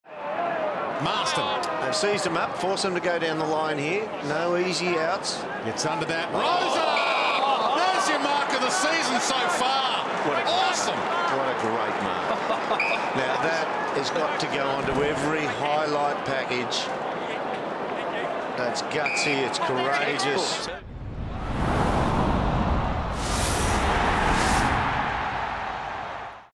AFL Mark of the Year - Matt Rosa Round 6 2014
West Coast's Matt Rosa is a round six Lifebroker Mark of the Year nominee.